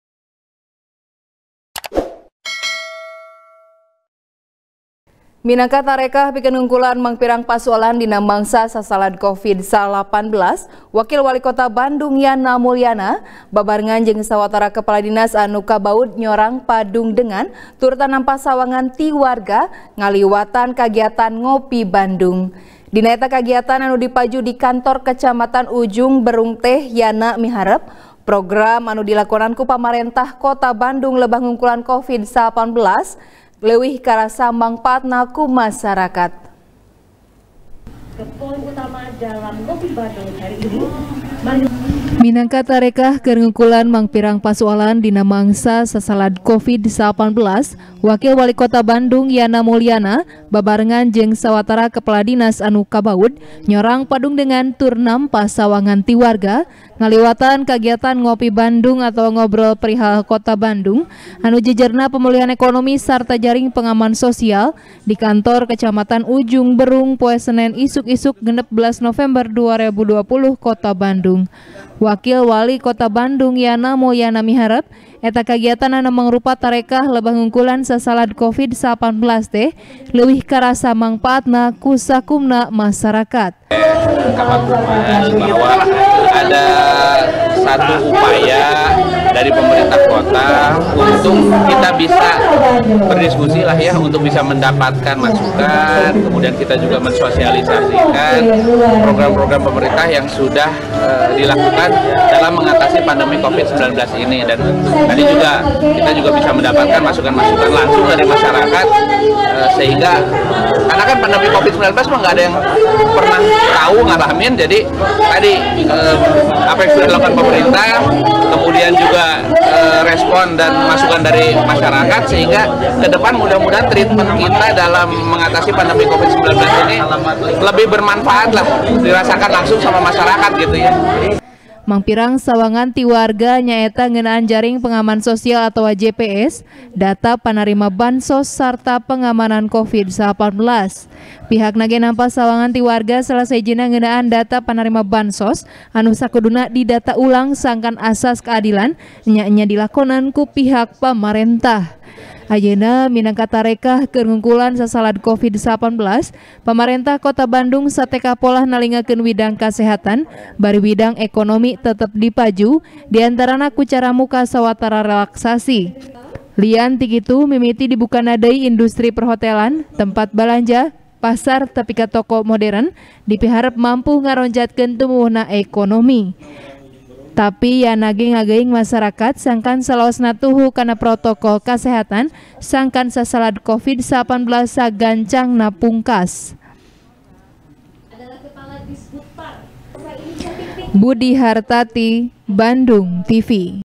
Yana Mulyana Padungdengan Dina Ngopi Bandung Ngeunaan Covid-19 | TP | BANDUNGTV
TP | BANDUNGTV | ELBY NOPITRI | Minangka Tarekah Keur Ngungkulan Mangpirang Pasualan Dina Mangsa Sasalad Covid-19, Wakil Walikota Bandung Yana Mulyana, Babarengan Jeung Sawatara Kepala Dinas Anu Kabaud, Nyorang Padungdengan Tur Nampa Sawangan Ti Warga, Ngaliwatan Kagiatan Ngopi Bandung. Dina Eta Kagiatan Anu Dipaju Di Kantor Kecamatan Ujung Berung Teh, Yana Miharep, Program Anu Dilakonan Ku Pamarentah Kota Bandung Lebah Ngungkulan Covid-19, Leuwih Karasa Mangpaatna Ku Warga.